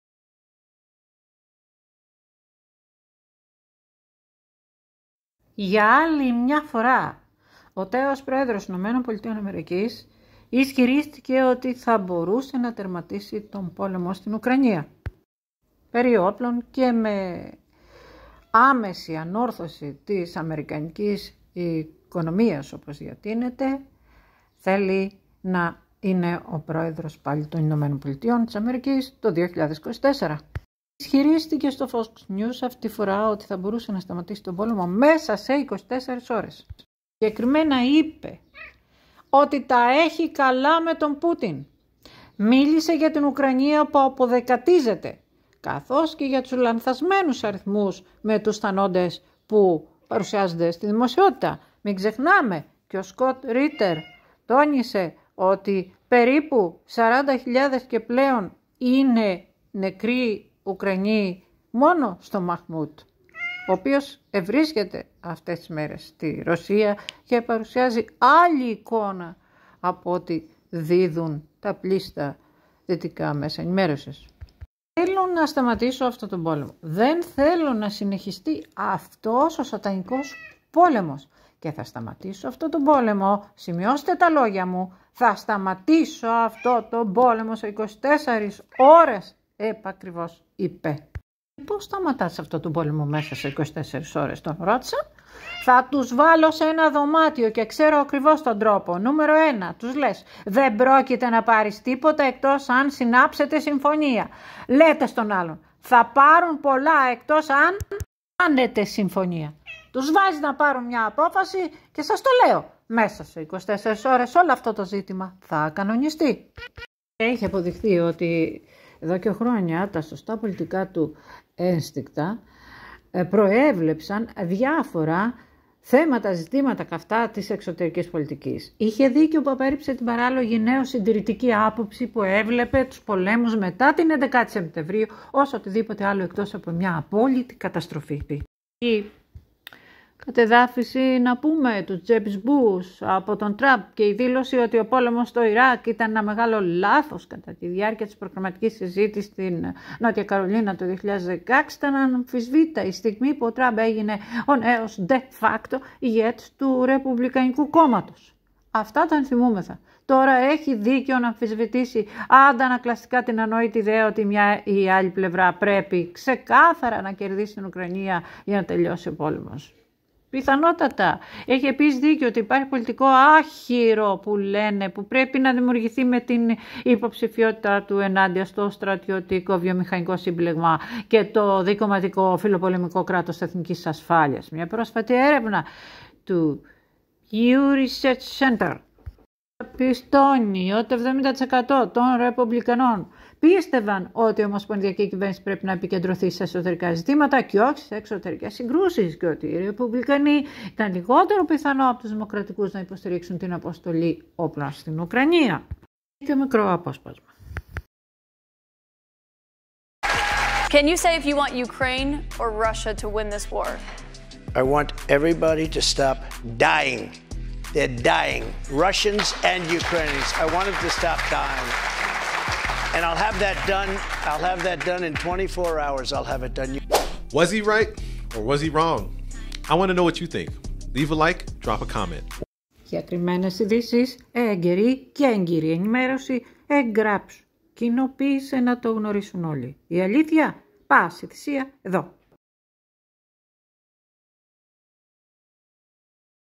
trump"Θα σταματήσω το πόλεμο,μέσα σε 24 ώρες." Aκούστε πως!
***Ακούστε και για τις πολιτικές προβλέψεις του τότε και σήμερα..